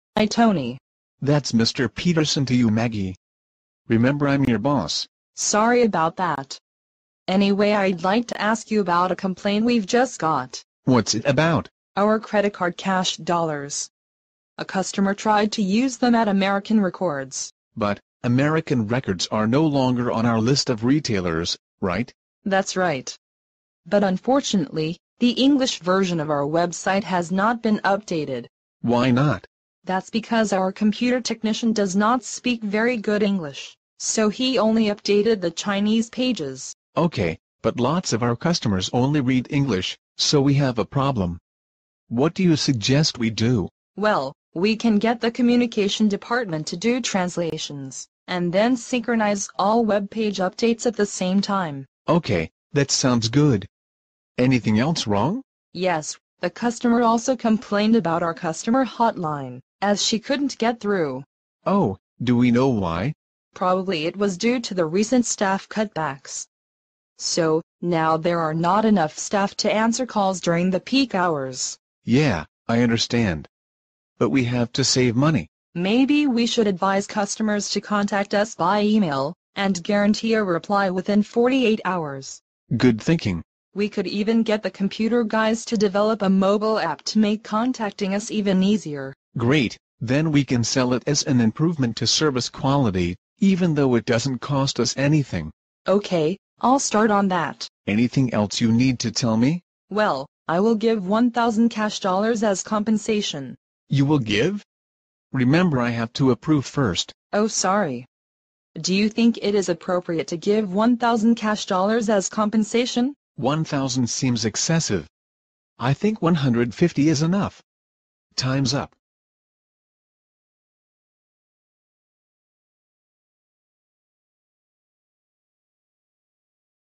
Solving Complaints in the Workplace
Maggie Lam tells Mr Peterson about the complaint Global Bank has received.

How would you solve it?

Click on captions for English subtitles.

R9.5